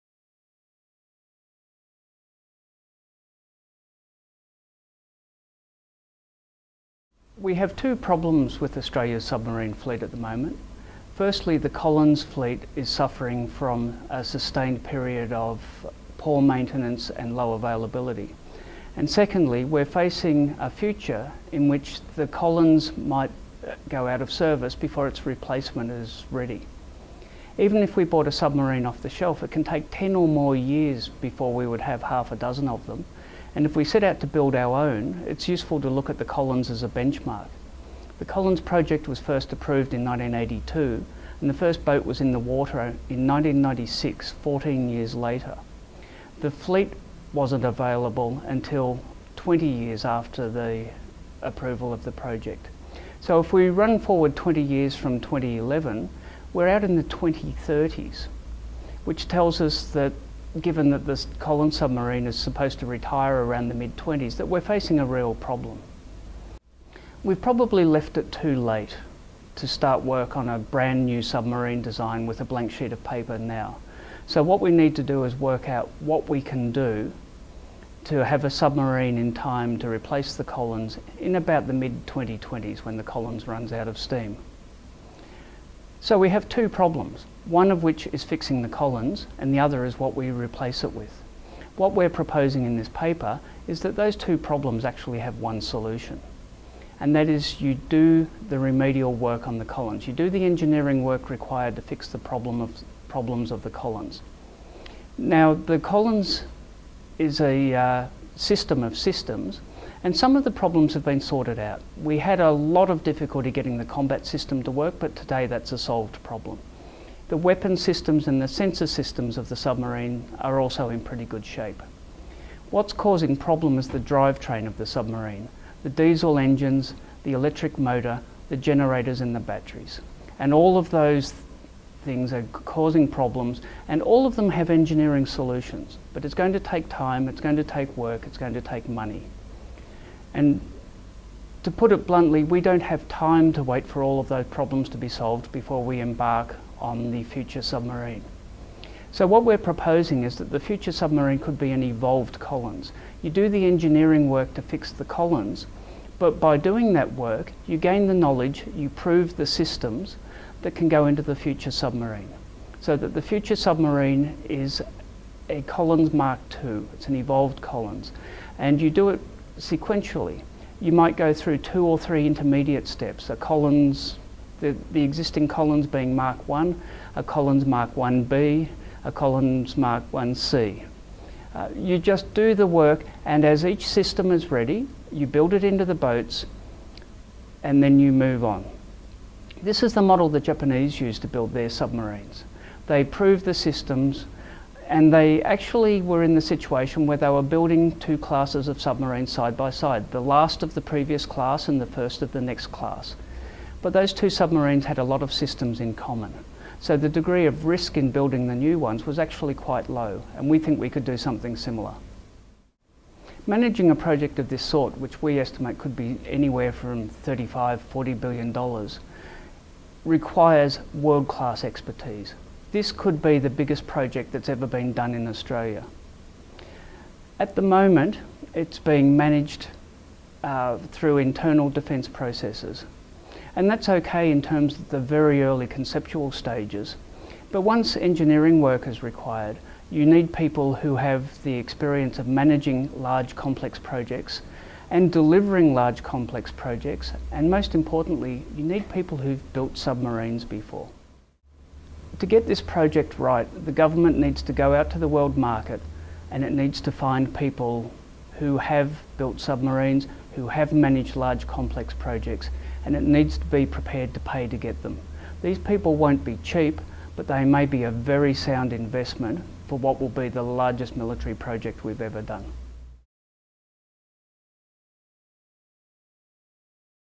The future evolution of Australia's submarines - Andrew Davies, ASPI
Andrew Davies speaks about his paper, "Subaqueana australis--the future evolution of Australia's submarines", available for download

The future submarine project is potentially the nation's biggest ever engineering undertaking. But at the moment it is making little progress. In part that's because the ongoing saga of the Collins fleet is making decision makers uncomfortable with the idea of 'throwing good money after bad'. And in part it's because the principals in charge of the project aren't able to make an authoritative case for the way ahead.

The national submarine capability needs to be managed holistically -- treating Collins and the future submarine as stand-alone problems increases the chance that there'll be a future capability gap between the two classes. Fixing the problems with the Collins and developing the technologies that will go into the future boats can be the same activity. At this stage an 'evolved Collins' might be the best bet. But whatever we decide to do, we need to get the best possible people into the top management jobs. And that means the best that the world market can provide—people with experience in delivering big industrial projects and, of course, building submarines.